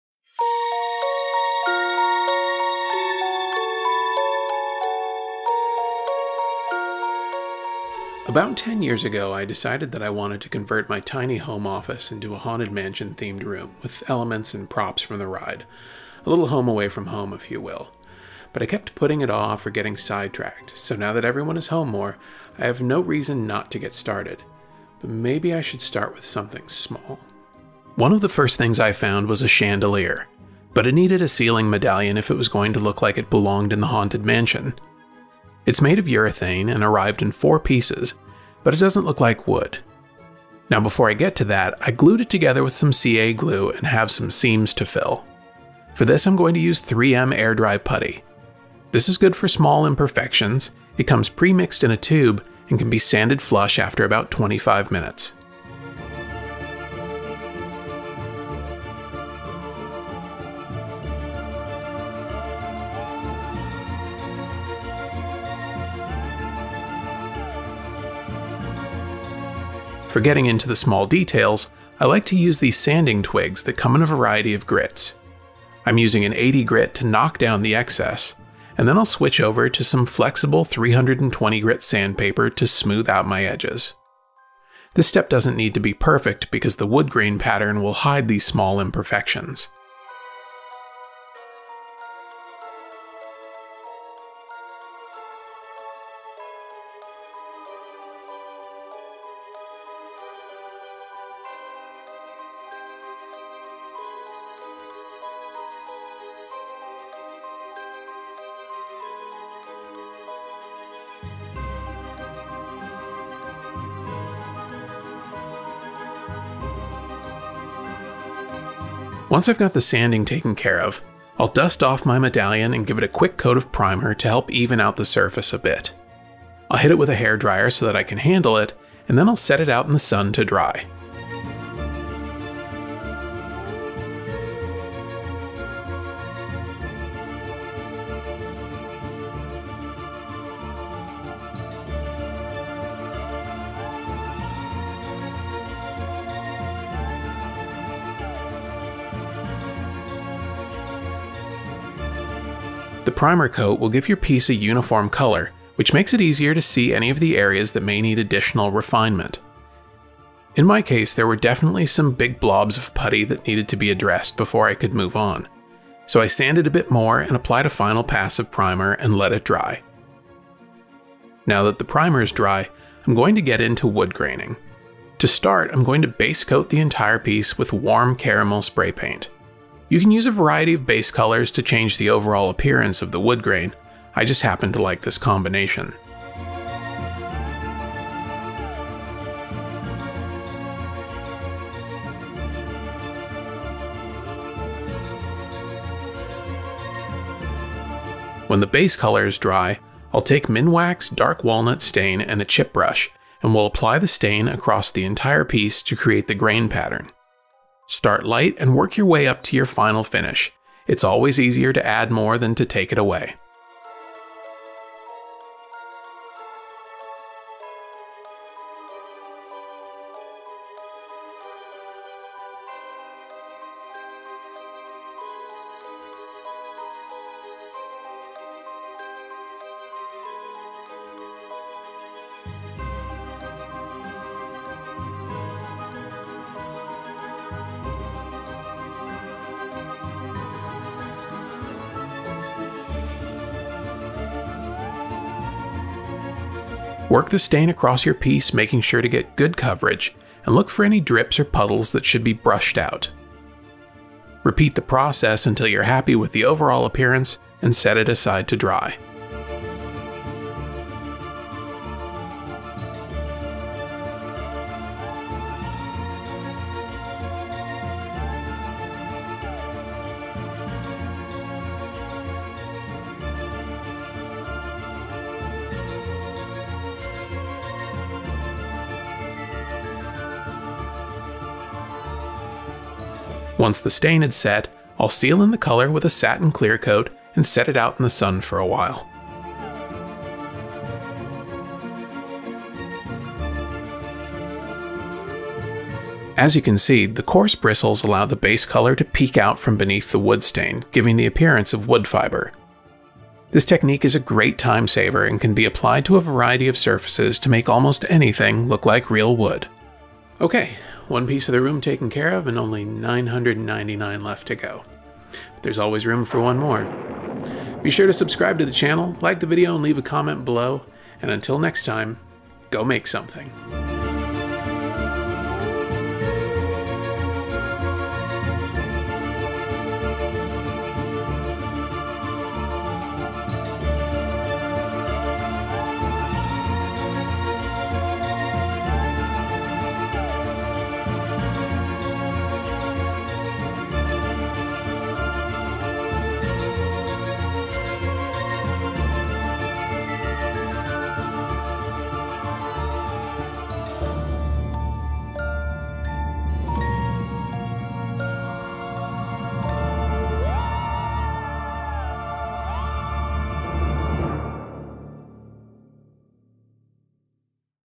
SERIES: Haunted Mansion Home Office Part 1 - Faux Wood Ceiling Medallion
Have you ever wanted a Haunted Mansion home office or bedroom? I have for about 10 years, but it always got put on the back burner until now. Join me as I turn my small home office into a mini Haunted Mansion starting with a chandelier and faux wood ceiling medallion.

Do you enjoy our episodes? Do you want a behind the scenes look and more?

SPECIAL THANKS:
YouTube.com/Wesleytreat
Instagram.com/wesleytreat

MATERIALS:
Candelabra sleeves
3M acryl putty
Sanding twigs
Flexible sandpaper
Rustoleum sandable filler primer
Chip Brush
Hair dryer

PAINTS*:
Crystal Clear Satin
Satin Warm Caramel
Minwax Dark Walnut 2716 Wood Stain